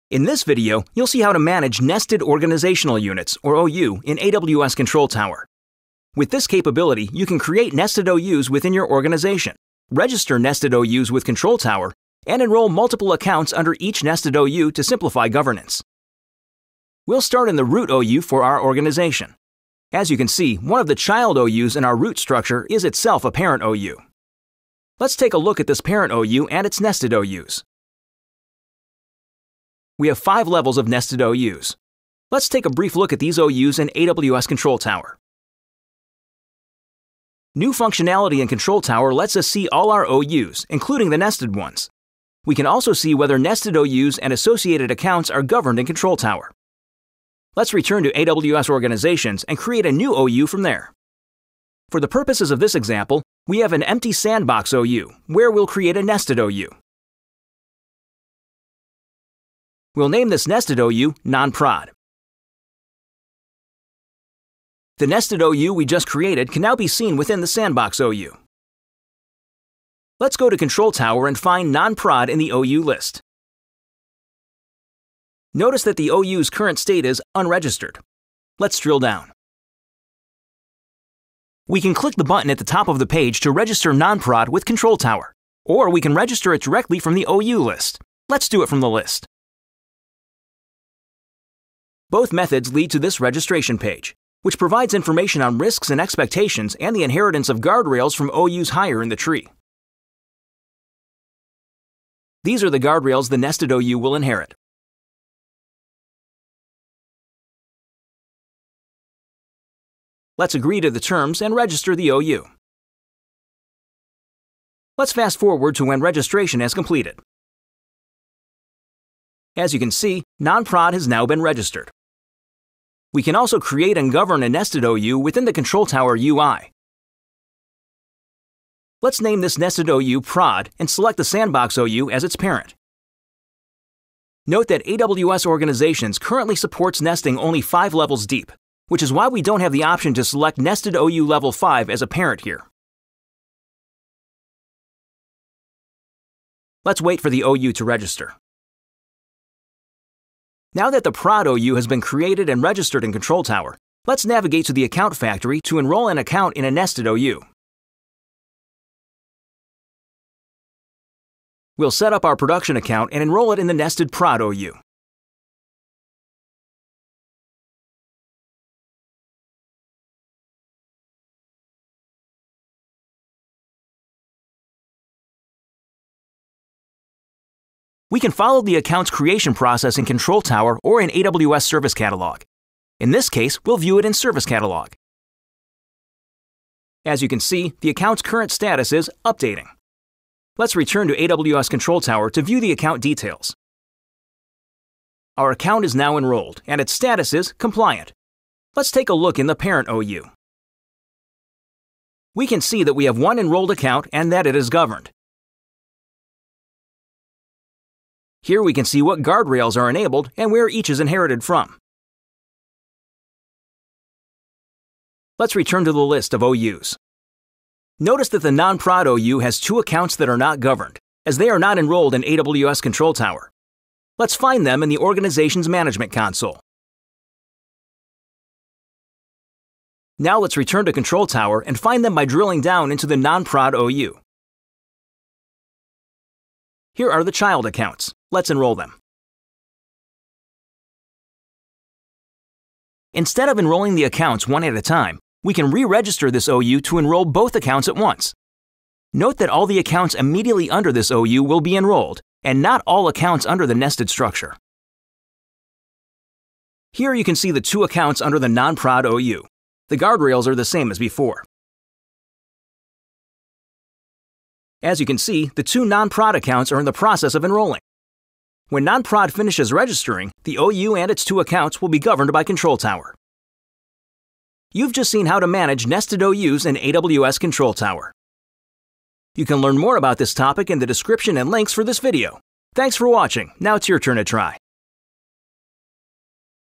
Manage Nested Organizational Units in AWS Control Tower | Amazon Web Services
In this video, you’ll see how to manage nested organizational units (OUs) in AWS Control Tower. With this capability, you can create nested OUs within your organization, register nested OUs with Control Tower, and enroll multiple accounts under each nested OU to simplify governance.